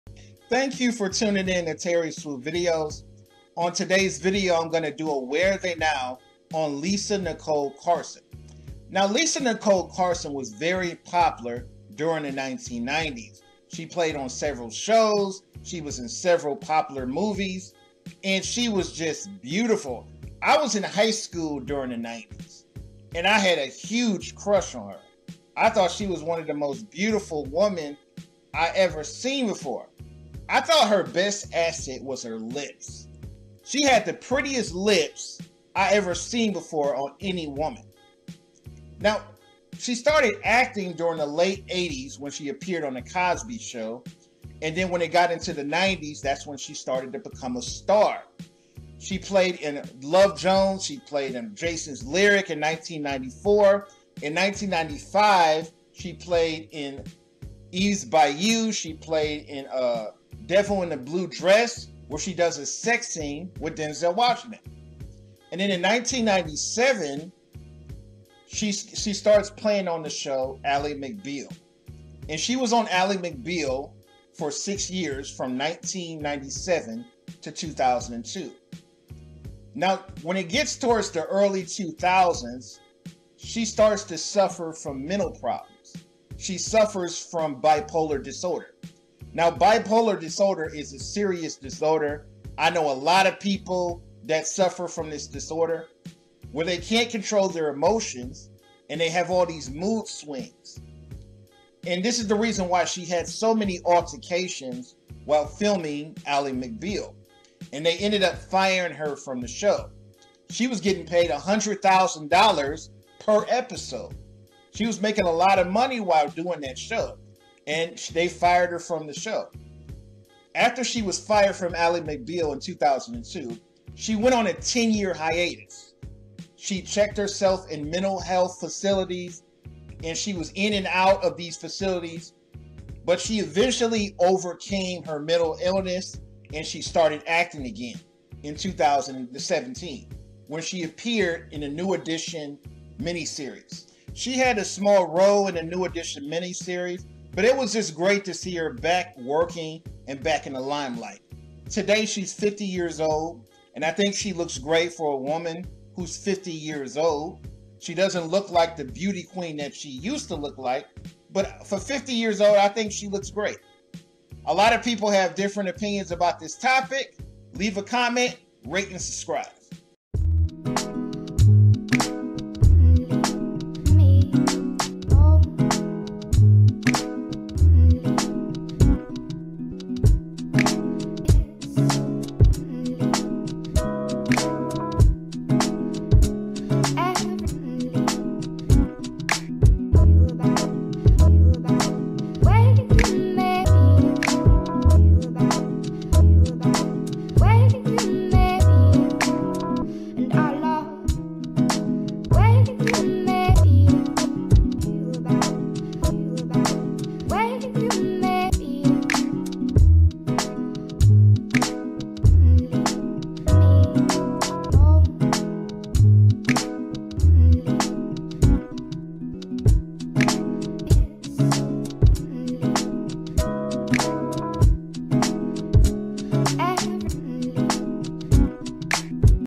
Remember 90's Beauty Lisa Nicole Carson
I give commentary addressing what happened to the actress Lisa Nicole Carson and what she's up to now.